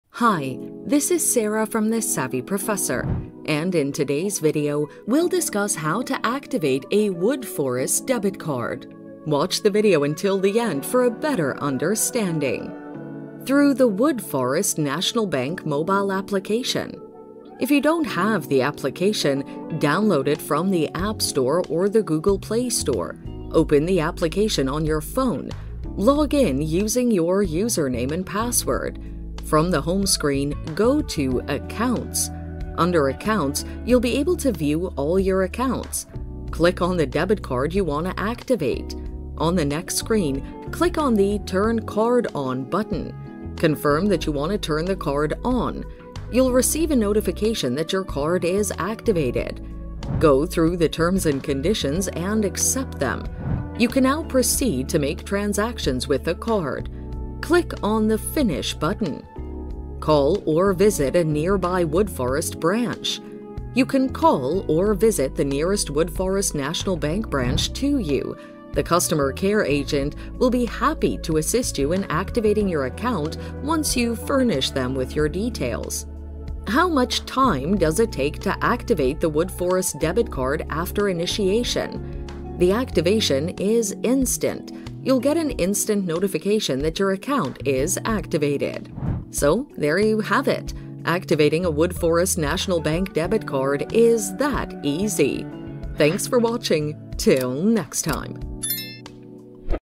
How To Activate Woodforest Debit Card (Instructions To Activate Woodforest Debit Card)
How To Activate Woodforest Debit Card (Instructions To Activate Woodforest Debit Card). In this video tutorial I will show you activate Woodforest debit card.

Through the Woodforest National Bank Mobile Application.

1. If you don't have the application, download it from the App Store or the Google Play Store.
2. Open the application on your phone.
3. Log in using your username and Password.
4. From the Home screen, go to Accounts.
5. Under Accounts, you will be able to view all your accounts.
6. Click on the debit card you want to activate.
7. On the next screen, Click on Turn Card on button.
8. Confirm that you want to turn the card on.
9. You will receive a notification that your card is activated.
11. You can now proceed to make transactions with the card.
12. Click on the Finish button.

Call or Visit a Nearby Woodforest branch.

You can call or visit the nearest Woodforest National Bank branch to you. The Customer care agents will be happy to assist you in activating your account once you furnish them with your details.

How much time does it take to activate the Woodforest debit card after initiation?

The activation is instant. You will get an instant notification that your account is activated.